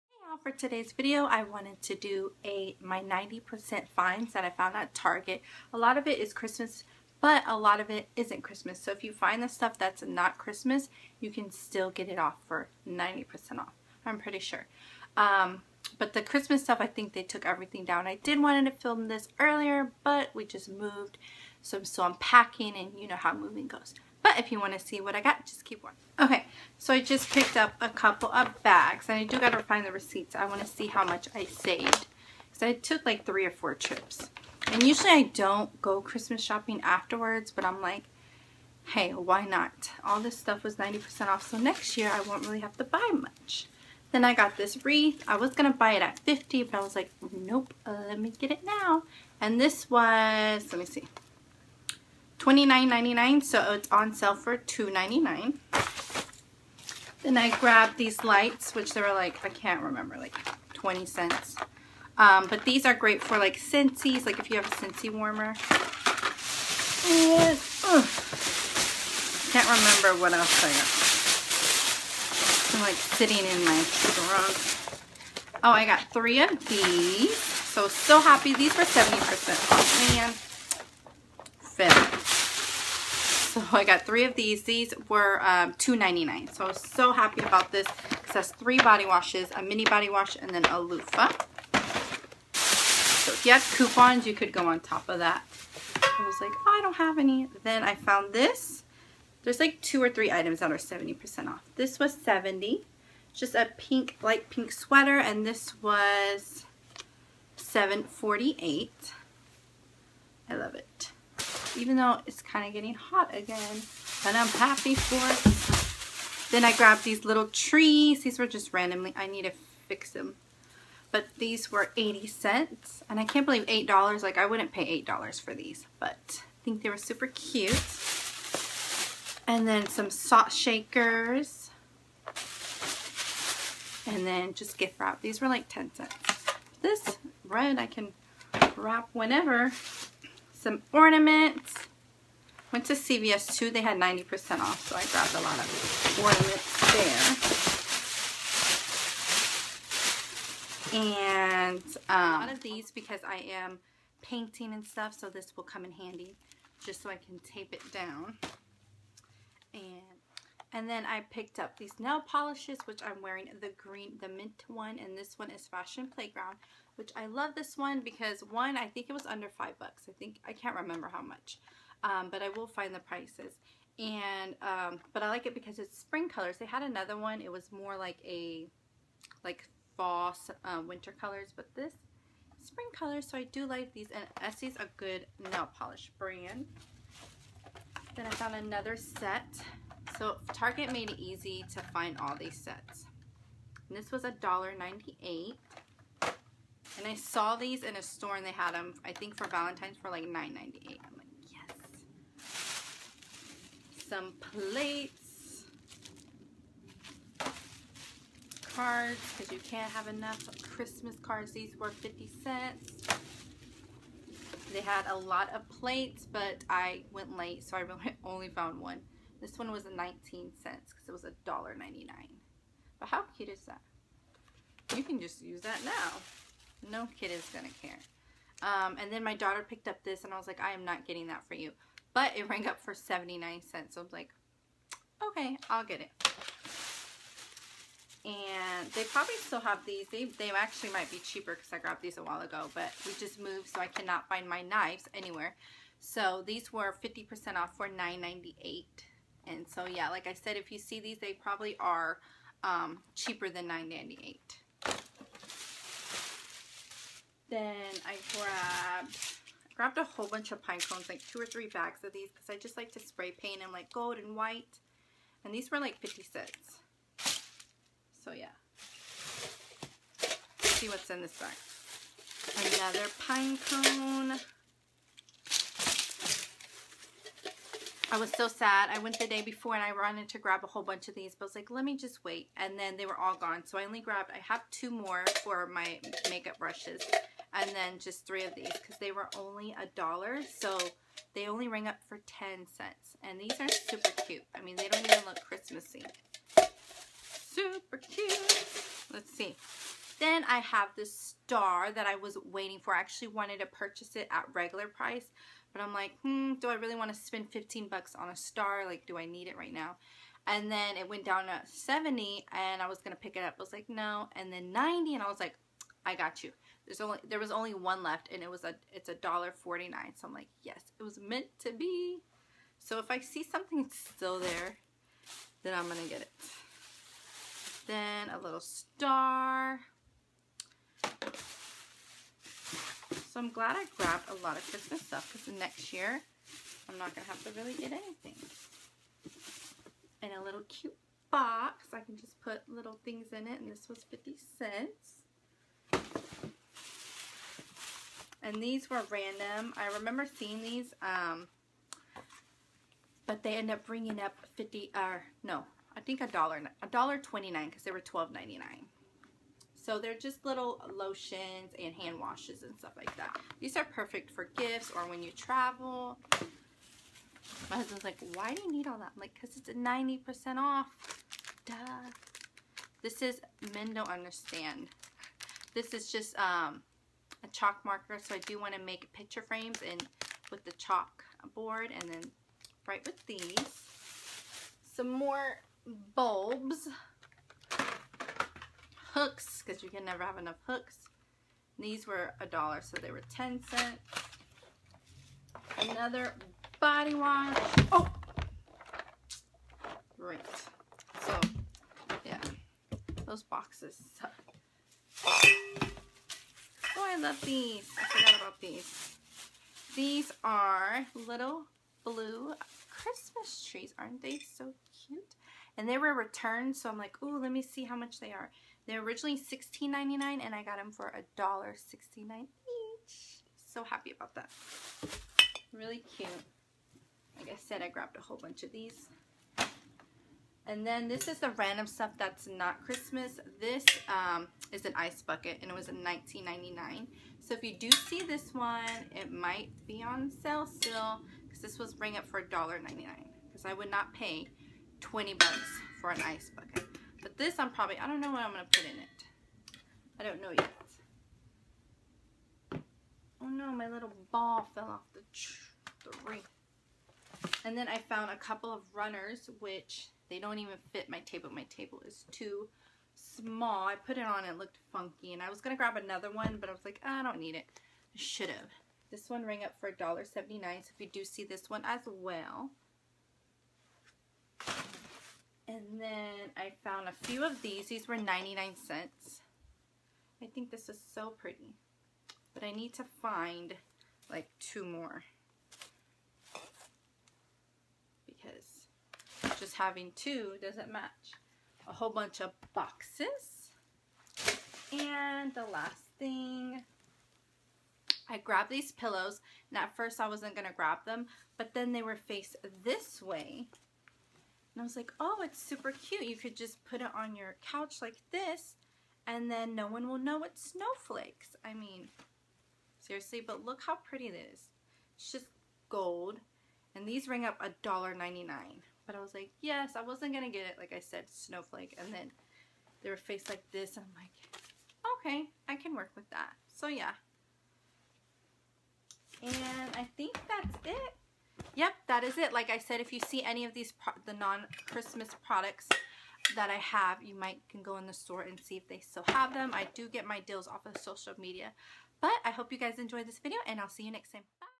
My 90% off Target Haul
I went to Target and found some awesome deals.
I usually find instagram